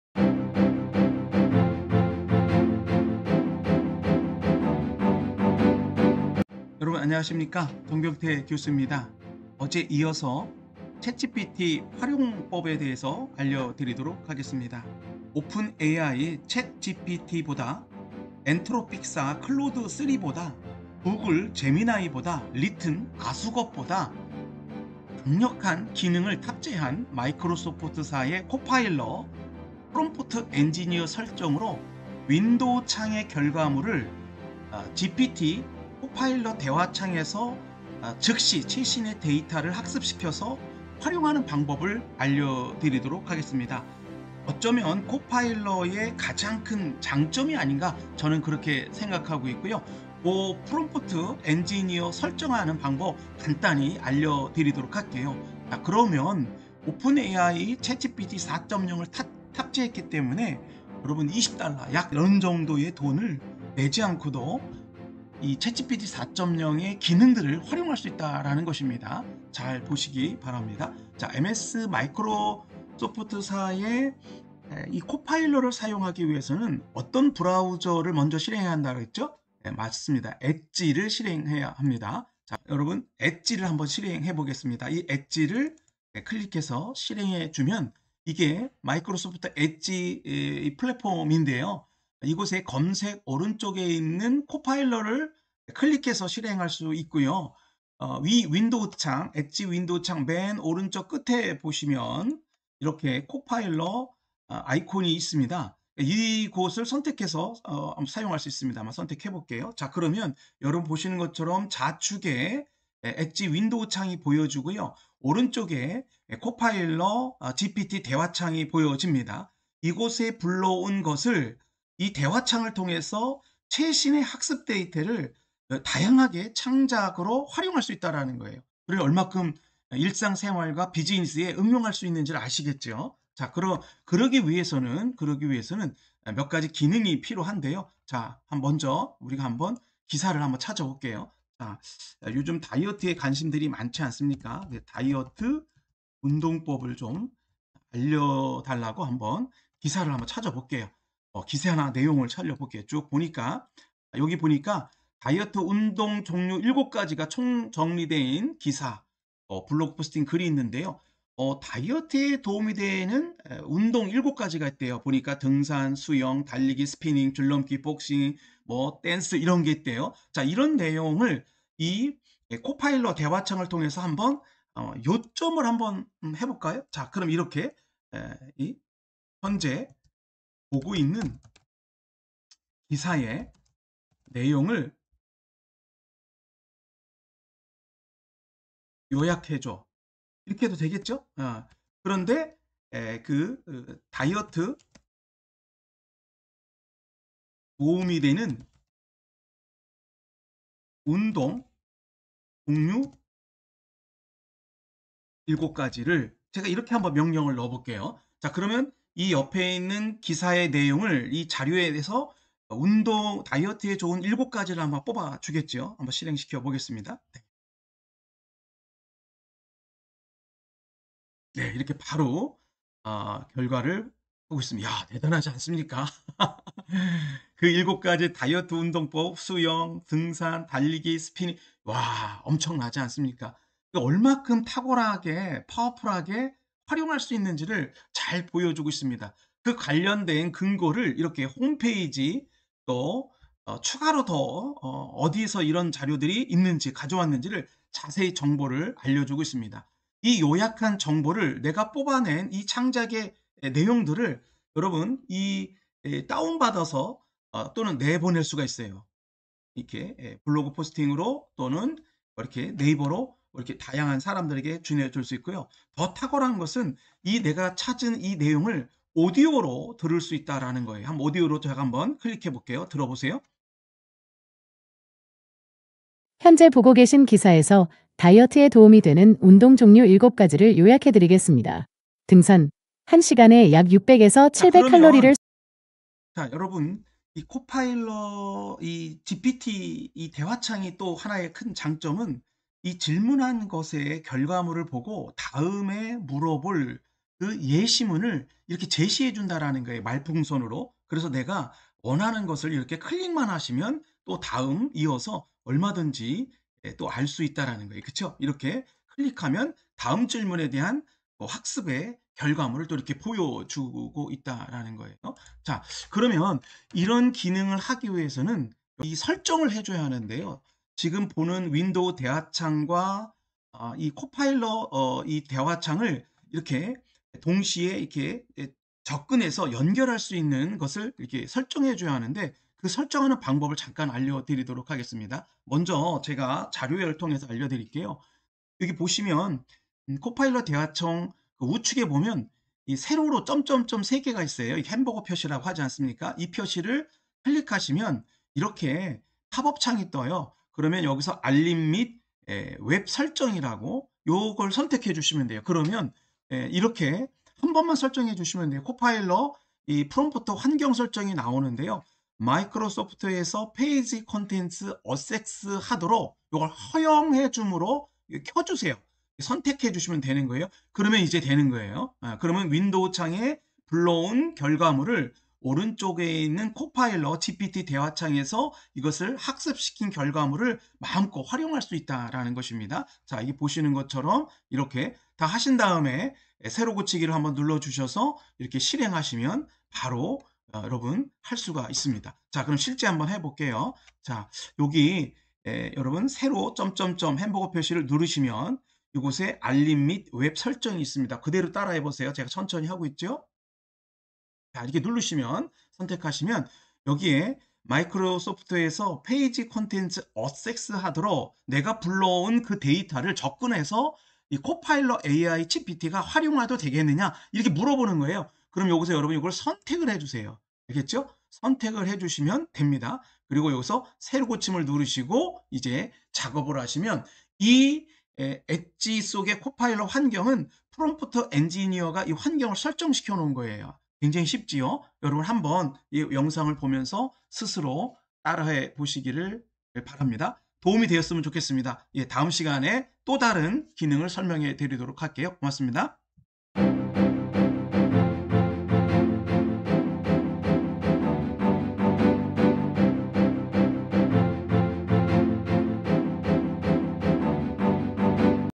그대로 따라만해도 챗GPT-4 무료 사용하기, 한 번의 설정으로 두 작업창 동시 활용
더 강력해진 코파일럿 강점
윈도우창과 코파일럿 창 동시에 활용 가능
한 번의 프롬프트 엔지니어 설정으로 챗GPT-4 무료 사용하기
정병태 교수의 챗GPT 기업 강의